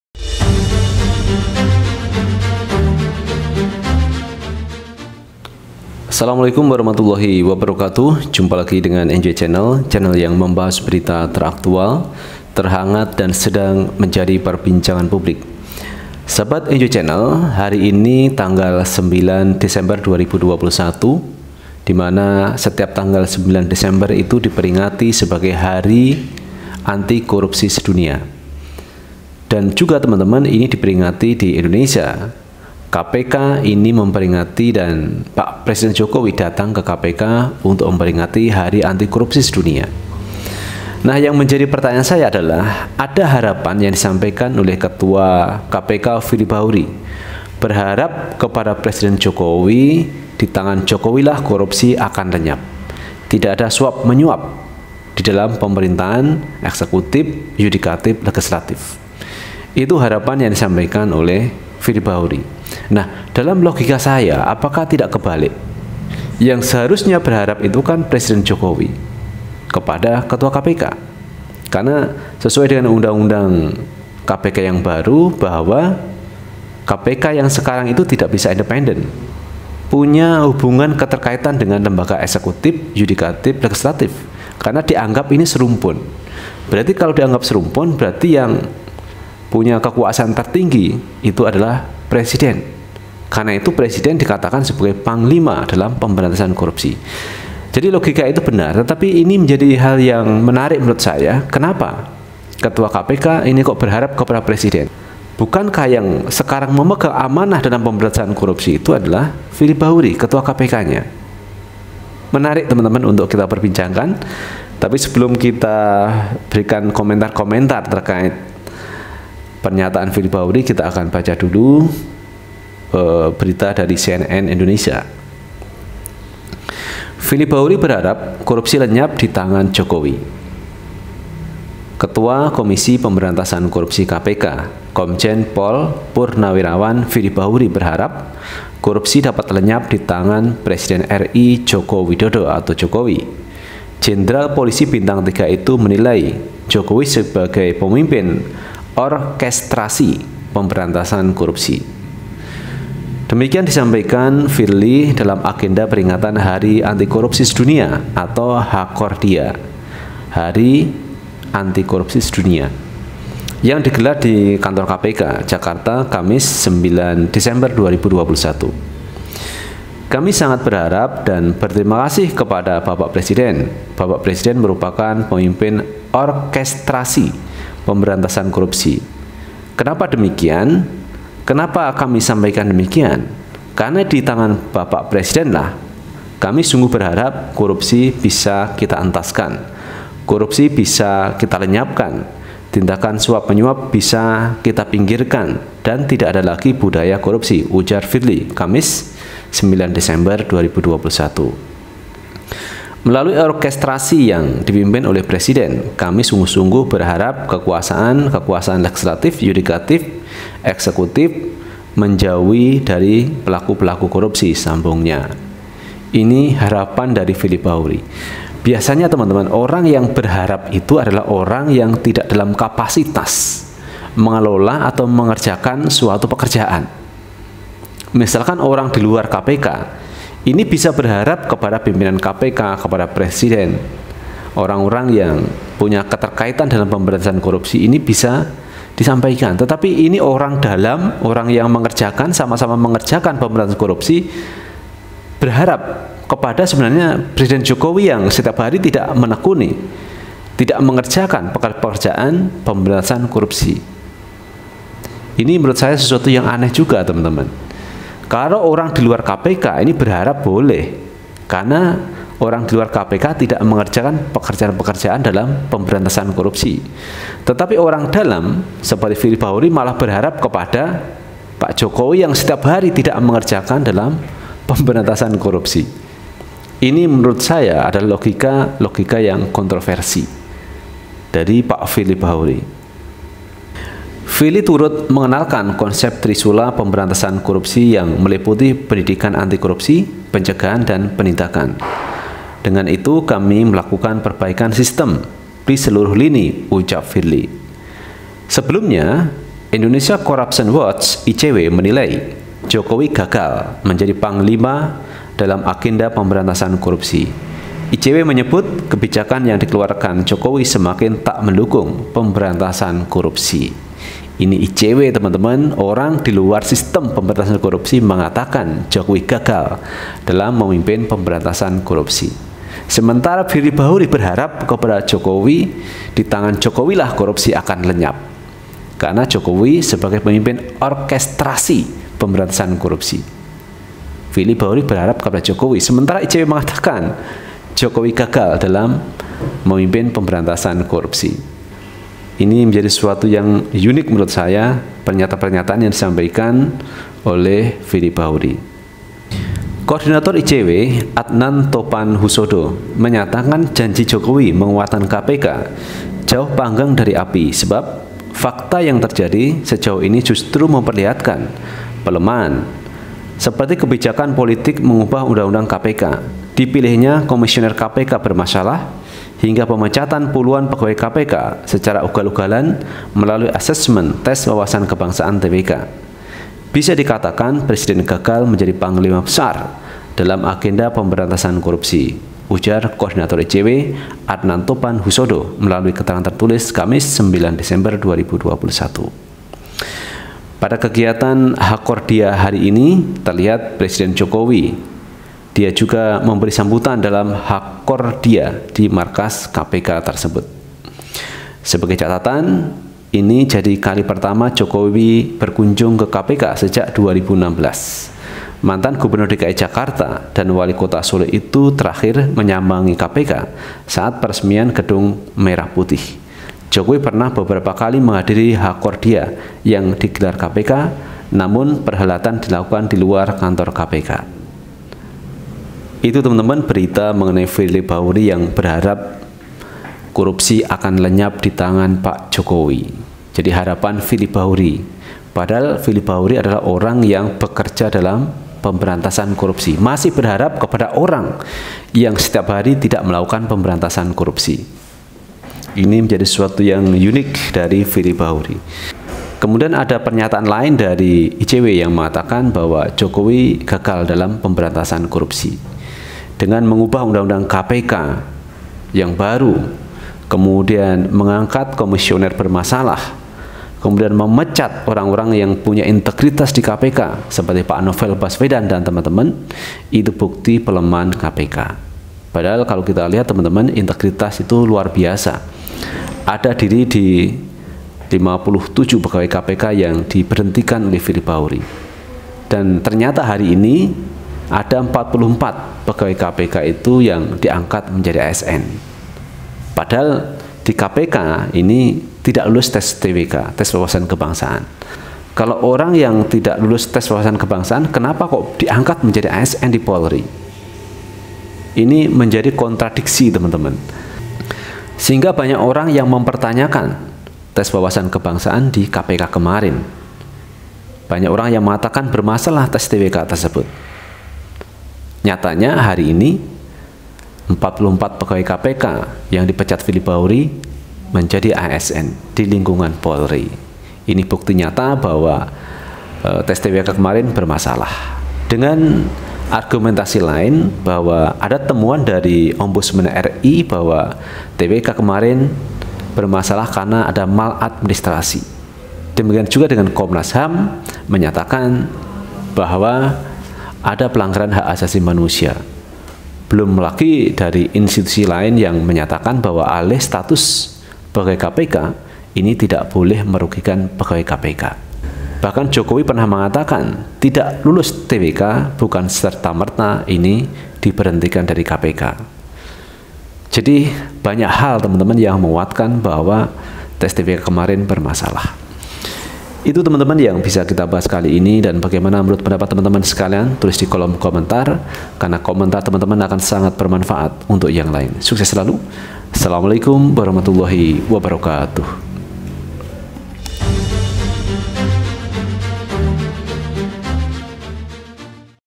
LUAR BIASA!! FIRLI BAHURI BERHARAP KOROPSI LENYAP DITANGAN JOKOWI, LHA YG JADI KPK NYA SIAPA ?
Enjoy Channel News, Jakarta - Ketua Komisi Pemberantasan Korupsi (KPK) Komjen Pol (Purn) Firli Bahuri berharap korupsi dapat lenyap di tangan Presiden RI Joko Widodo (Jokowi). Jenderal polisi bintang tiga itu menilai Jokowi sebagai pemimpin orkestrasi pemberantasan korupsi.

Demikian disampaikan Firli dalam agenda peringatan Hari Antikorupsi Sedunia (Hakordia) yang digelar di Kantor KPK, Jakarta, Kamis (9/12).

"Kami sangat berharap dan berterima kasih kepada Bapak Presiden, Bapak Presiden merupakan pemimpin orkestrasi pemberantasan korupsi. Kenapa kami sampaikan demikian? Karena di tangan Bapak Presiden lah kami sungguh berharap korupsi bisa kita entaskan, korupsi bisa kita lenyapkan, tindakan suap-menyuap bisa kita pinggirkan dan tidak ada lagi budaya korupsi," ujar Firli, Kamis (9/12).